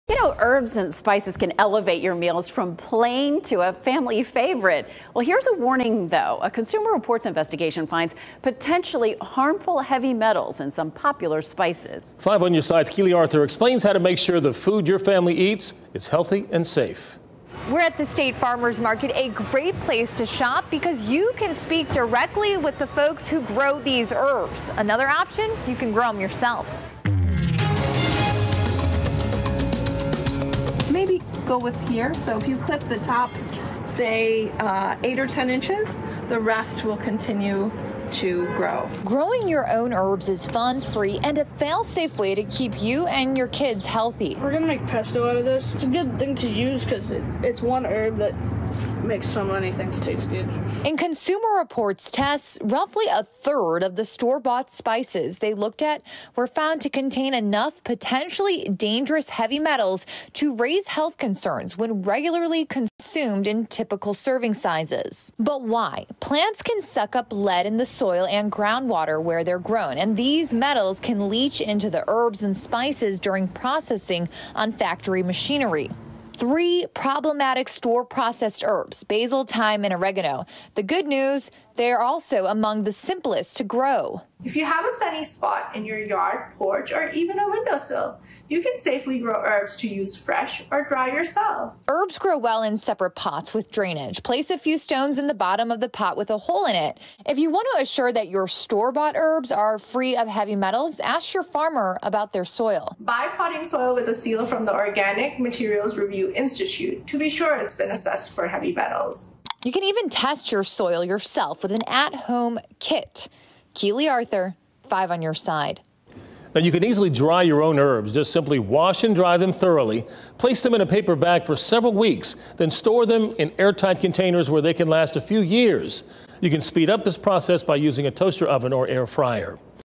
Grow your own herb garden: Tips for growing, drying herbs and spices to avoid toxic metals
Herbs and spices can elevate your meals from plain to a family favorite. But here's a warning: A Consumer Reports investigation found potentially harmful heavy metals in some popular spices. Here's more on the spices to watch out for and an easy way to avoid heavy metals. .